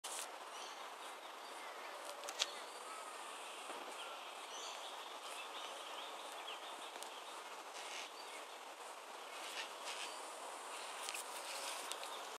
Yellow tail black cockatoo taking flight
Short clip of a yellow tail black cockatoo taking flight. Earlier it flew past 2 metres above our heads. Wish I got that on camera!